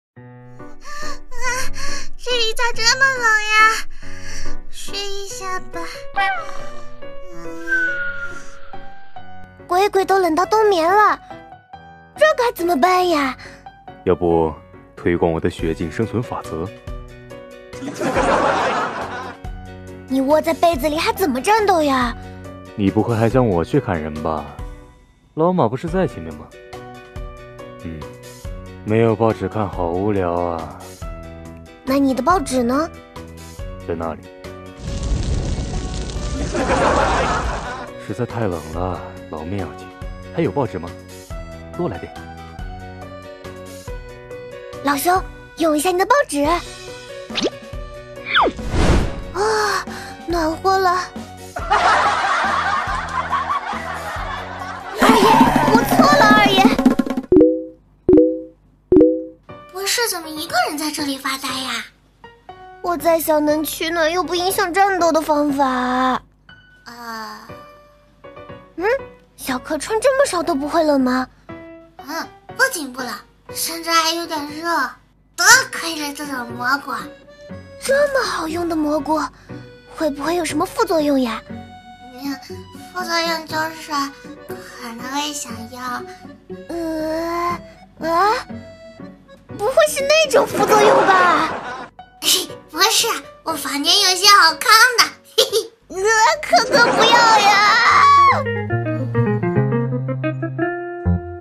【Arknights】Ceobe on drugs (again)
Arknights but everyone is just trying to survive in Kjerag. I can indeed confirm the dangers of frost, the closest I ever came to dying was when I visited Canada. Nearly froze to death.

The zombie you see here is the Newspaper Zombie from Plants vs Zombies. If you don't know, Mjolnir has been compared to the Newspaper Zombie before due to, well, the fact that they both read newspapers. In this video he is actually referred to by a nickname (according to a comment I read the nickname was due to how strong he was in PvZ), which I have omitted for simplicity.

On the later part when Ceobe comes in it is an obvious reference to the Fungimist event. The "side effect" that is being insinuated is of a sexual nature. At the end she actually refers to her as "刻哥" (something along the lines of "Brother Kay"), which is a reference to the "杰哥不要啊" video. Angelina's nickname of "洁哥" is also based on that video. By the way, "刻哥" is not an actual Ceobe nickname, it's just something from the video. If you are wondering what this "杰哥不要啊" video is about, I will not explain it because it is one of those things that you are better off not knowing.

Some parts simplified/modified for the sake of simplicity and to make it sound good.

CREDITS:

Original Video Title: 干员们会怎样面对谢拉格的严寒。【岛德罗321】